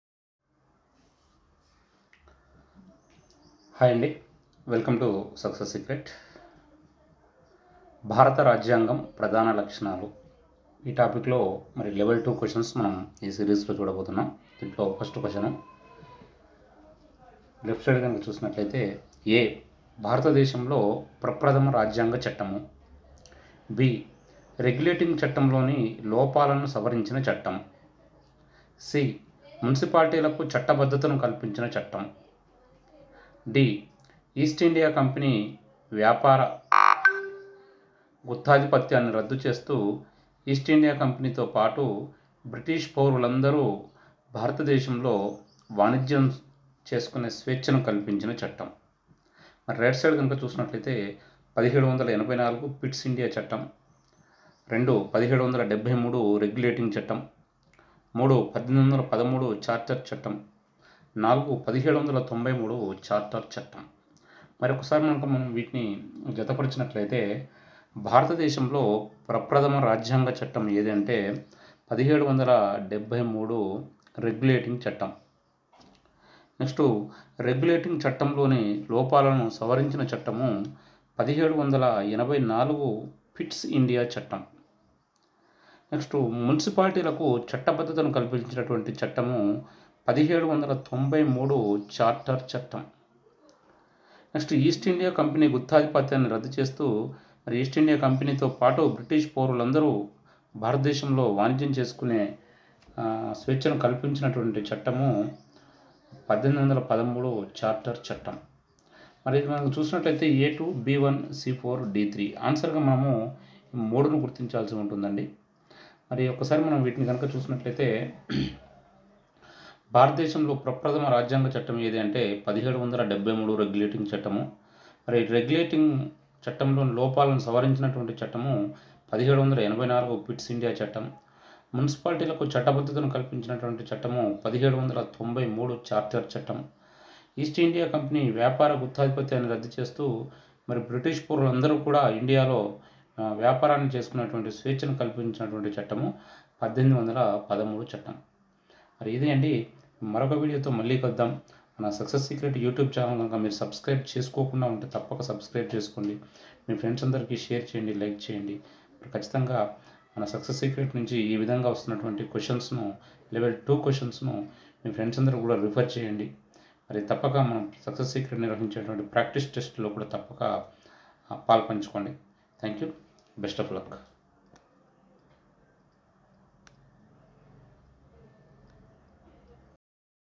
భారత రాజ్యాంగం : ప్రధాన లక్షణాలు -01 (Level-2) II success secret II
Indian Constitution : Salient Features

1.  Evolution of Indian Constitution – Nature and Salient Features – Preamble.
2.  Fundamental Rights – Directive Principles of State Policy – Fundamental Duties.
3.  Distinctive Features of Indian Federalism – Distribution of Legislative and Administrative Powers between Union and States.
4.  Union and State Governments – President – Prime Minister and Council of Ministers; Governor, Chief Minister and Council of Ministers – Powers and Functions.
5.  Rural and Urban Governance with special reference to the 73rd and 74th Amendments.
6.  Electoral System: Free and Fair Elections, Malpractices; Election Commission; Electoral Reforms and Political Parties.
7.  Judicial System in India – Judicial Activism.
8. a)  Special Provisions for Scheduled Castes, Scheduled Tribes, Backward Classes, Women and Minorities
b)  Welfare Mechanism for Enforcement – National Commission for  Scheduled Castes, National Commission for Scheduled Tribes and National Commission for Backward Classes.
9.  Indian Constitution: New Challenges.